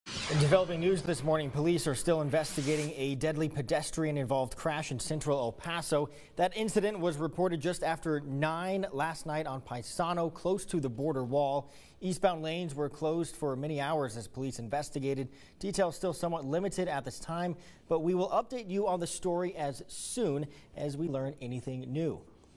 1 person dead in pedestrian crash in Central El Paso
One person is dead after a pedestrian crash Wednesday evening, July 19 in Central El Paso, according to El Paso Police.